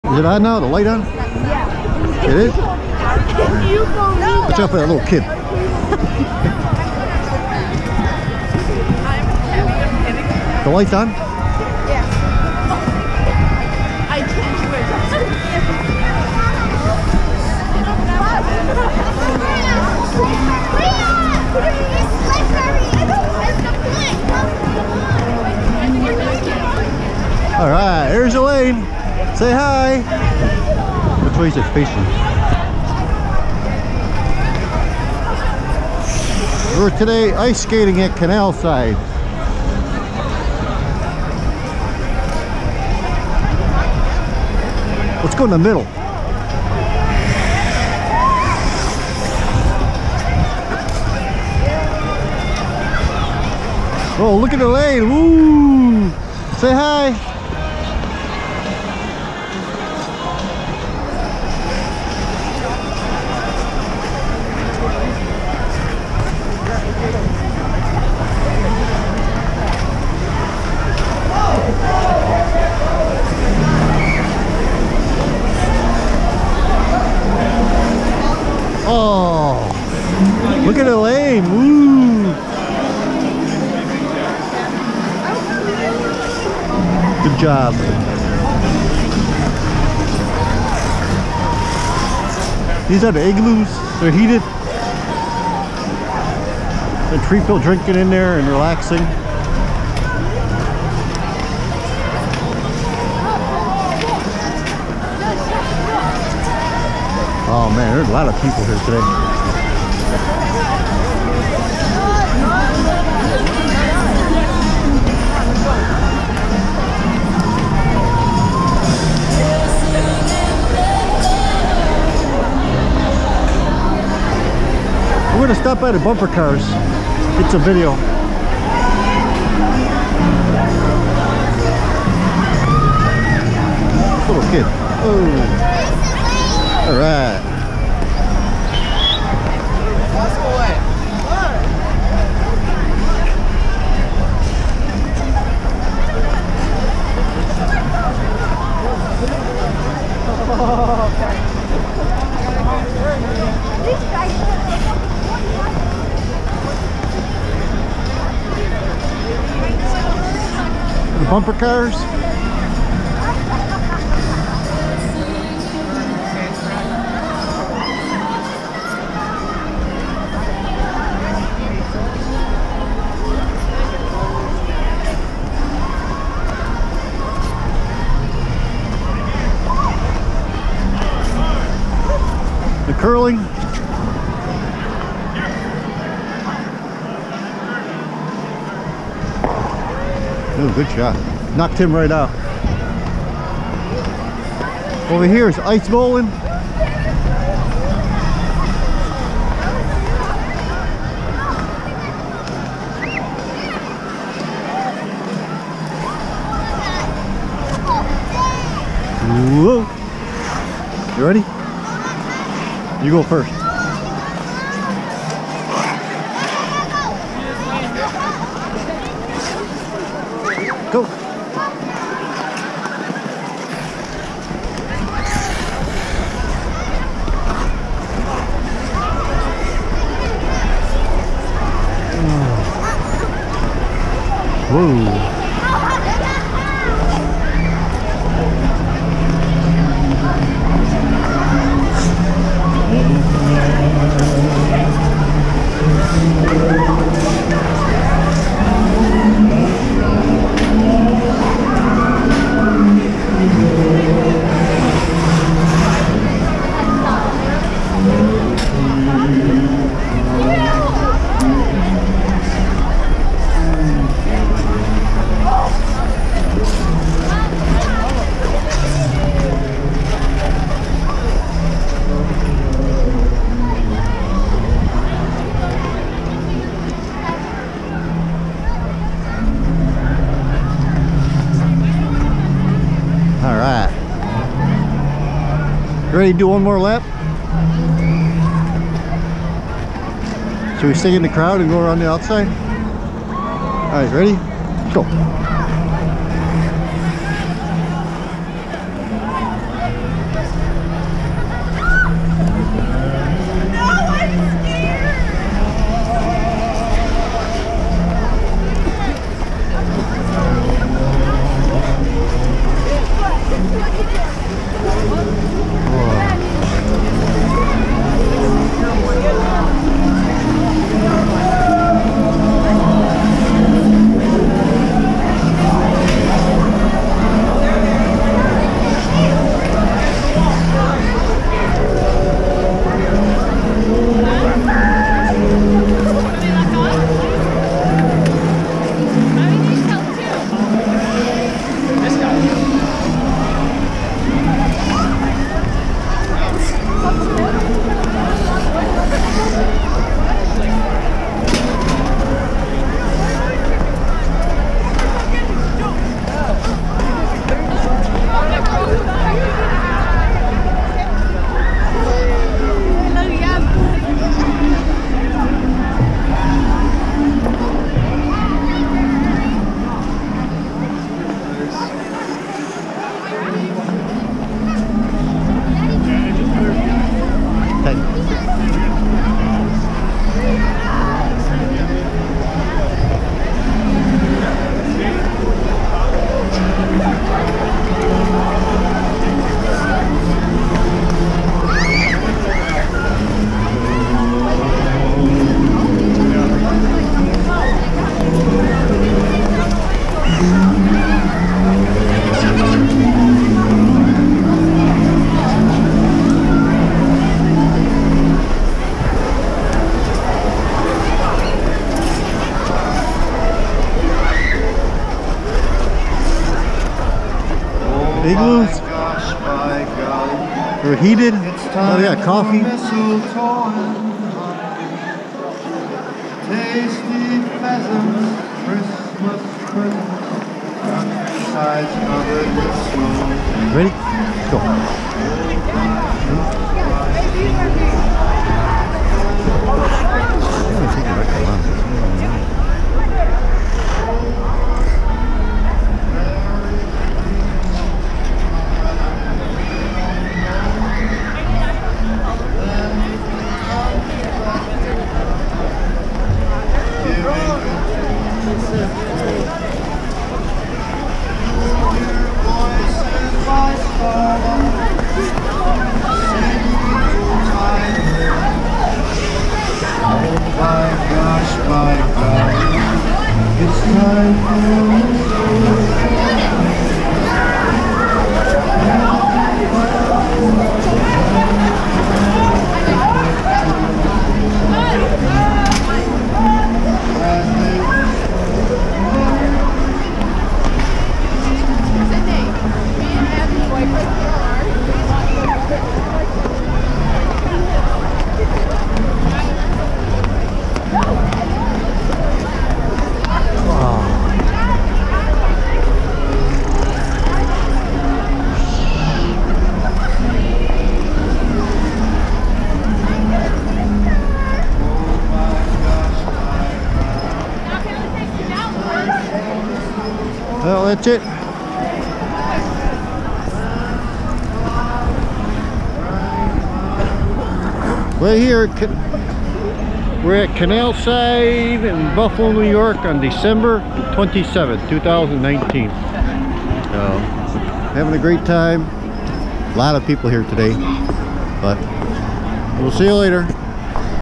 Ice Skating Canalside Buffalo New York.Dec 27,2019
I went ice skating today with my beautiful daughter Elaine.There was alot of skaters tonight but fun.The Igloo cost 190.00 for rent but you get drinks and food.We have our own skates.Good times and memories.